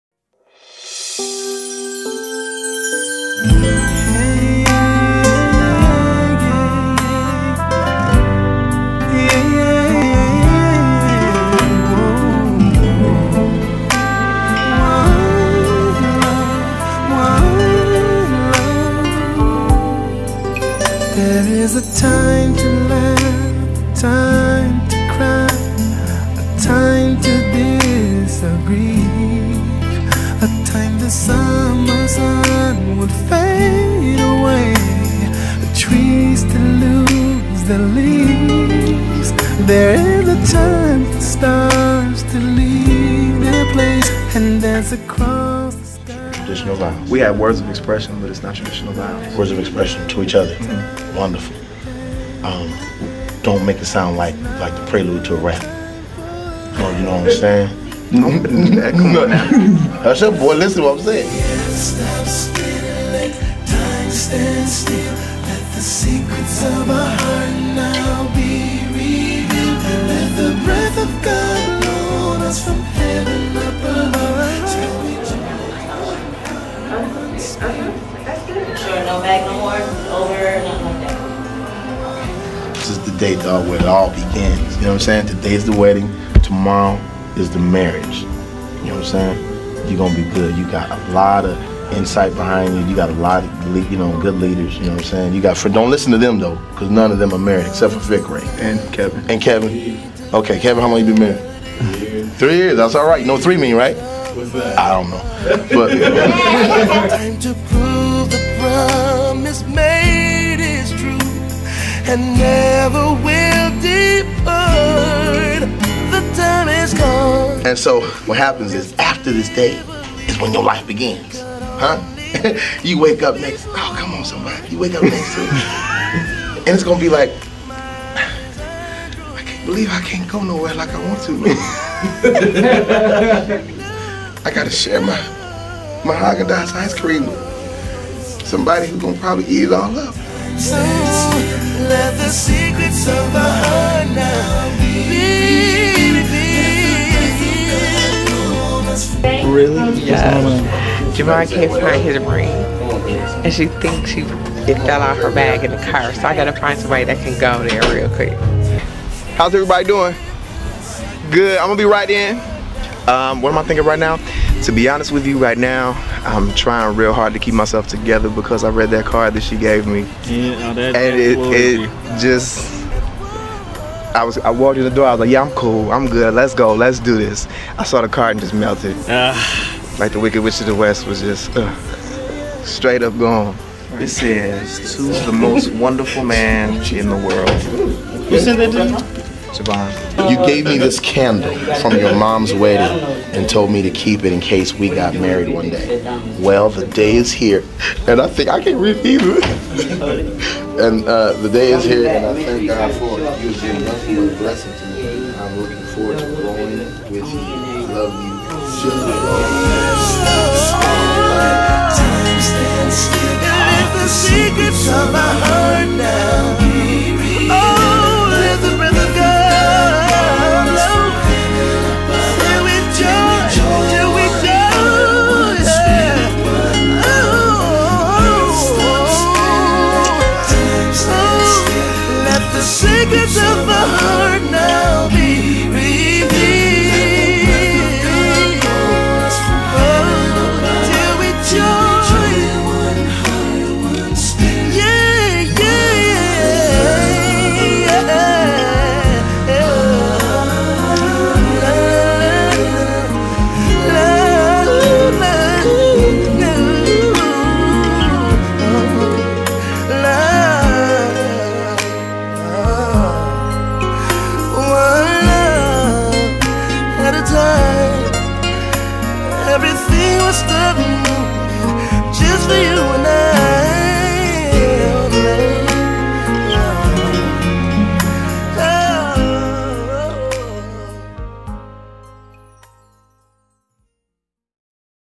Our Wedding Story: Keith and JaVon Jason (Pre-Wedding)
Footage from just prior to the wedding.